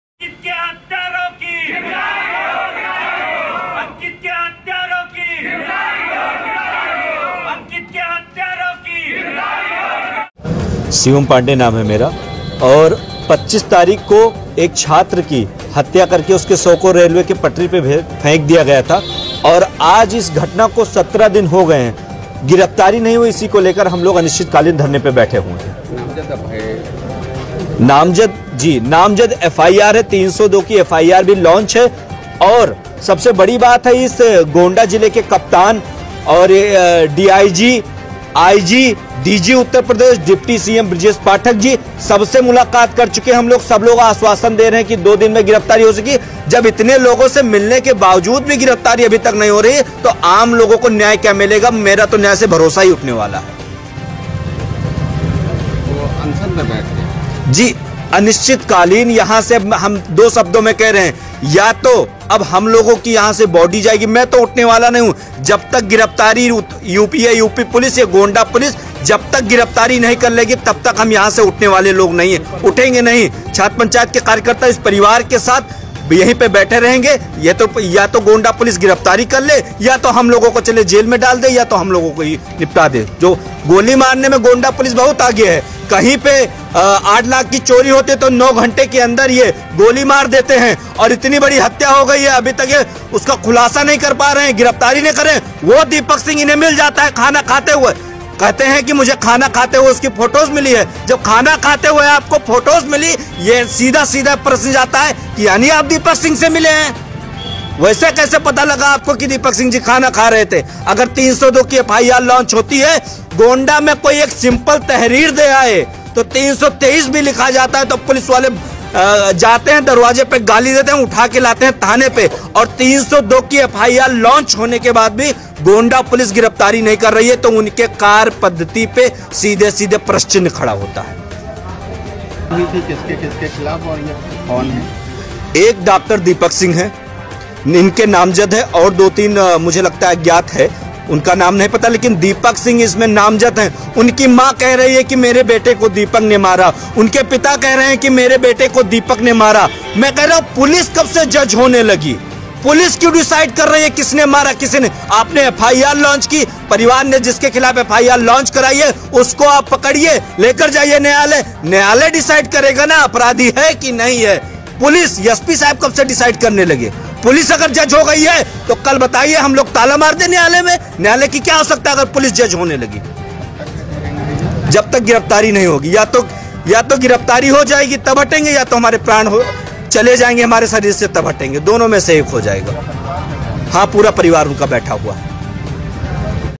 अंकित तिवारी के हथियारों की गिरफ्तारी की मांग को लेकर धरने पर बैठे शिवम पांडे...
DIRECTOR/EDITOR-N.K.VERMA